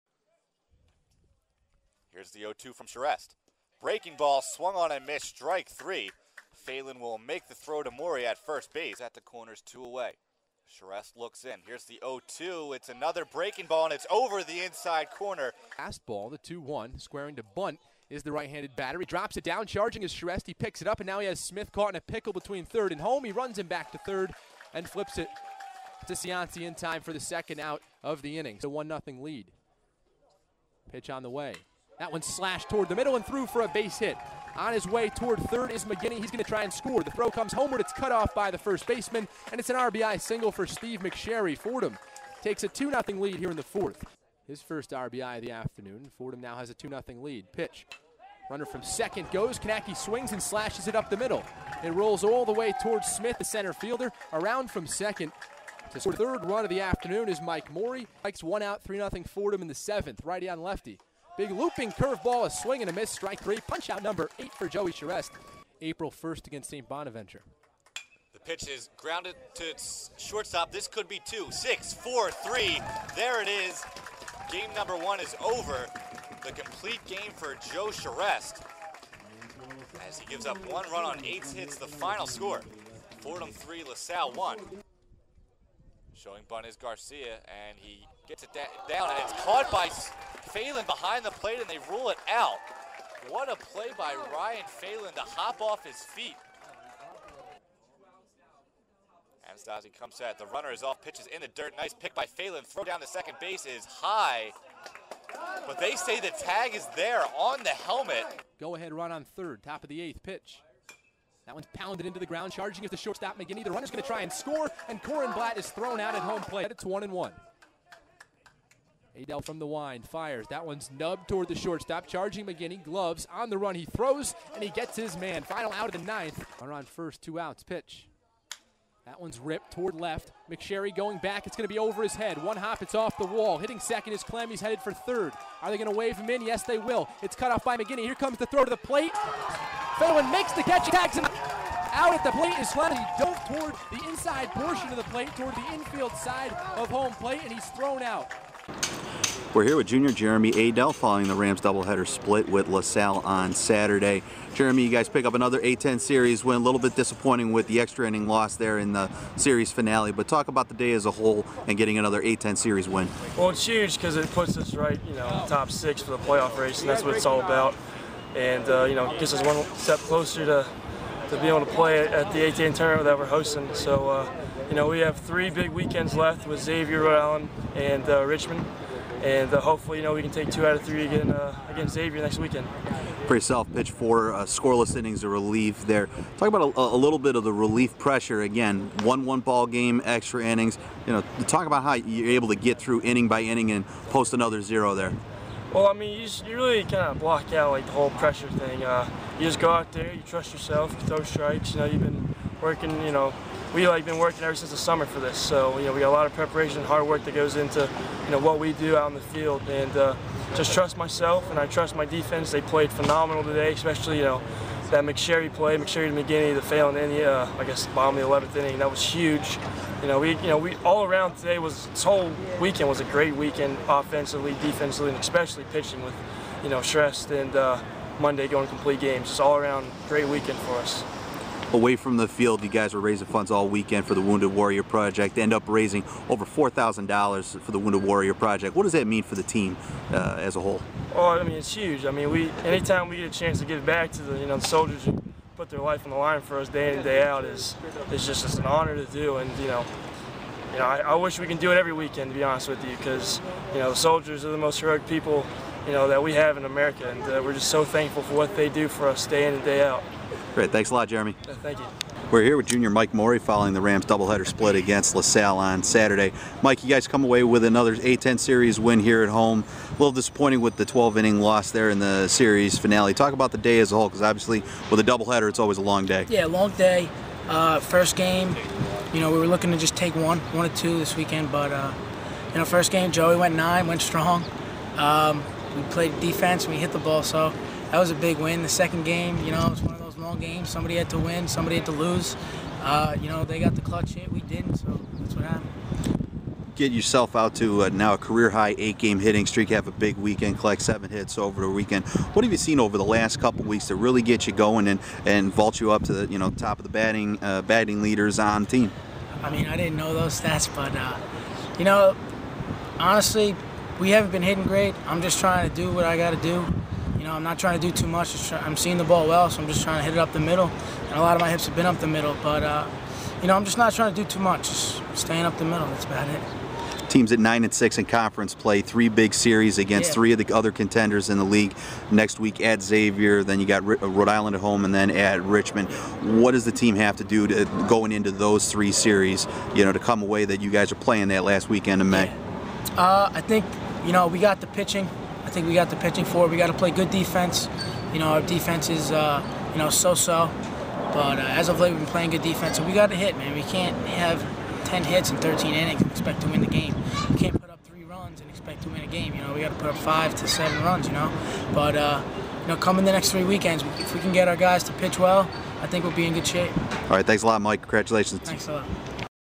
Fordham vs. La Salle Baseball Highlights - April 21, 2012
Fordham vs. La Salle Baseball Doubleheader Highlights & Interviews from April 21, 2012